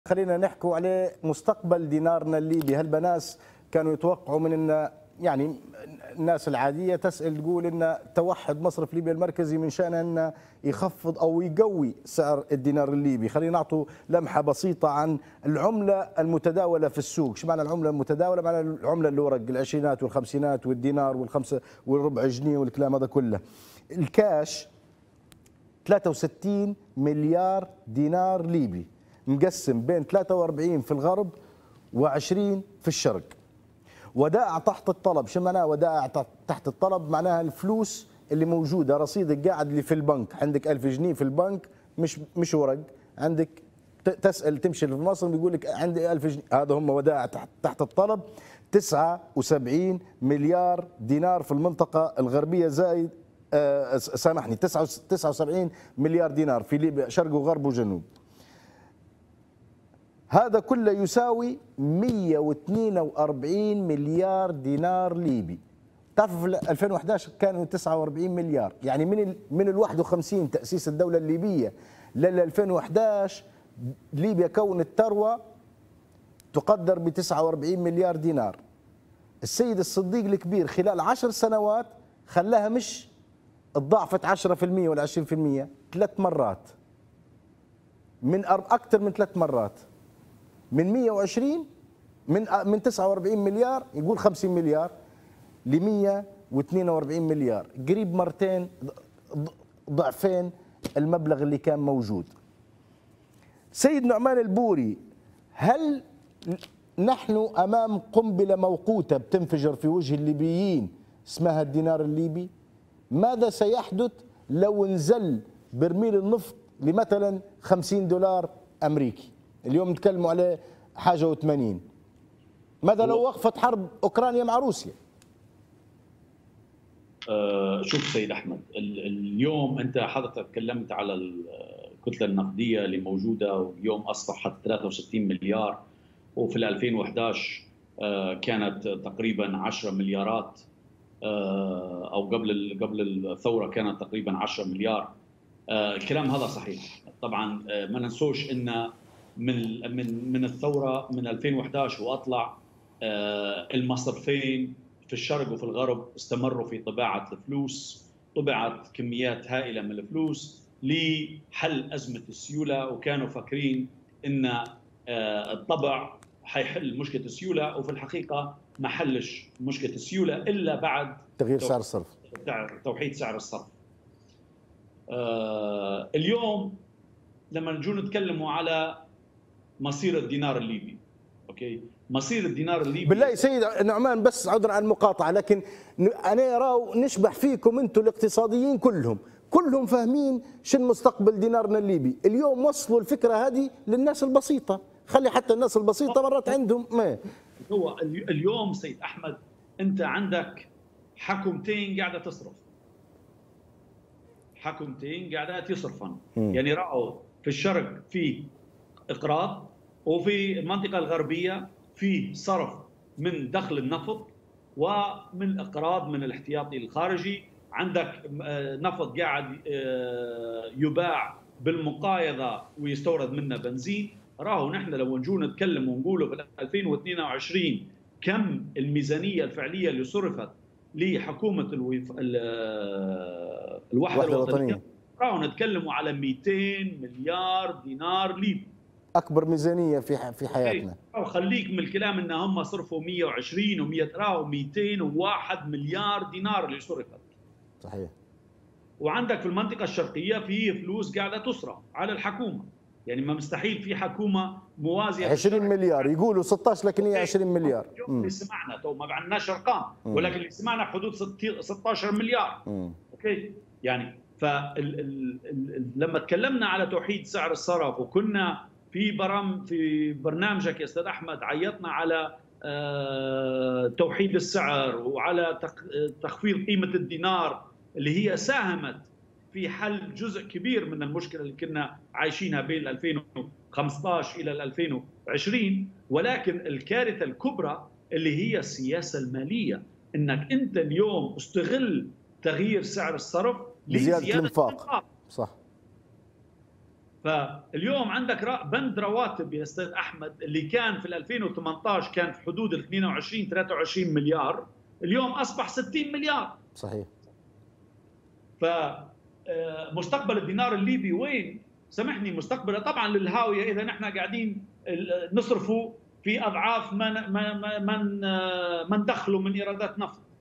كيف سيكون مستقبل دينارنا الليبي؟ وهل الدينار الليبي قنبلة موقوتة؟أحمد السنوسي بالأرقام يشرح بالأرقام
كيف سيكون مستقبل دينارنا الليبي ؟ وهل الدينار الليبي قنبلة موقوتة؟
أحمد السنوسي بالأرقام يشرح بالأرقام والمصرفي نعمان البوري يحلل :